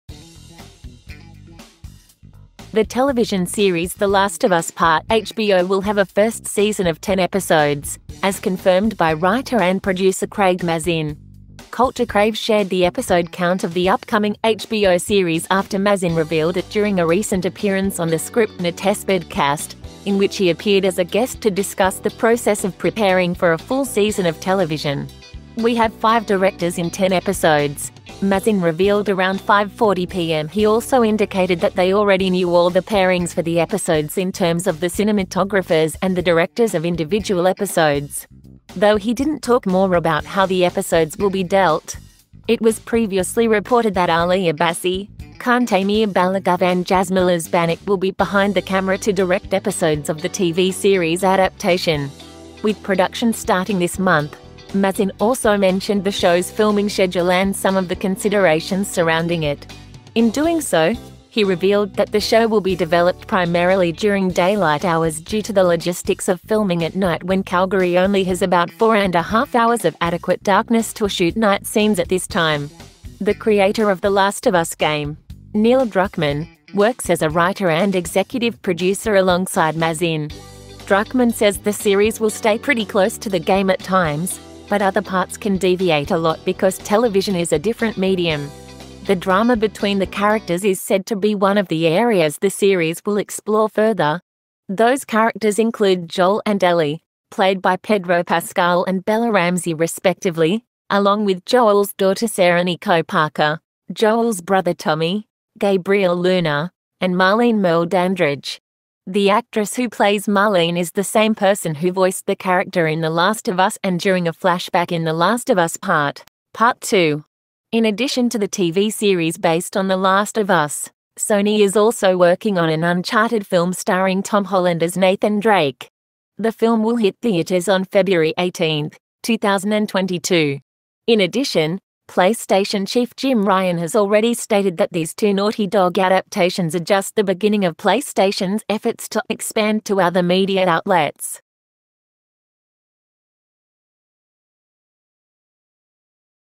New details revealed from season 1 of The Last of Us series for HBO
The show's writer and producer, Craig Mazin, has confirmed how many episodes the first season will have and more details about the show

The television series The Last of Us pa HBO will have a first season of 10 episodes,as confirmed by writer and producer Craig Mazin.

Culture Crave shared the episode count of the upcoming HBO series after Mazin revealed it during a recent appearance on the Scriptnotespodcast, in which he appeared as a guest to discuss the process of preparing for a full season of television. "We have five directors in 10 episodes,"Mazin revealed around 5:40 p.m.

He also indicated that they already knew all the pairings for the episodes in terms of the cinematographers and the directors of individual episodes, though he didn't talk more about how the episodes will be dealt. It was previously reported that Ali Abbasi, Kantemir Balagov and Jasmila Zbanic will be behind the camera to direct episodes of the TV series adaptation.

With production starting this month, Mazin also mentioned the show's filming schedule and some of the considerations surrounding it. In doing so, he revealed that the show will be developed primarily during daylight hours due to the logistics of filming at night when Calgary only has about "4 1/2 hours of adequate darkness" to shoot night scenes at this time.

The creator of The Last of Us game, Neil Druckmann, works as a writer and executive producer alongside Mazin. Druckmann says the series will stay pretty close to the game at times, but other parts can "deviate a lot" because television is a different medium. The drama between the characters is said to be one of the areas the series will explore further.

Those characters include Joel and Ellie, played by Pedro Pascal and Bella Ramsey respectively, along with Joel's daughter Sarah (Nico Parker), Joel's brother Tommy (Gabriel Luna) and Marlene (Merle Dandridge). The actress who plays Marlene is the same person who voiced the character in The Last of Us and during a flashback in The Last of Us Part - Part 2.

In addition to the TV series based on The Last of Us, Sony is also working on an Uncharted film starring Tom Holland as Nathan Drake. The film will hit theaters on February 18, 2022. In addition, PlayStation chief Jim Ryan has already stated that these two Naughty Dog adaptations are "just the beginning" of PlayStation's efforts to expand to other media outlets..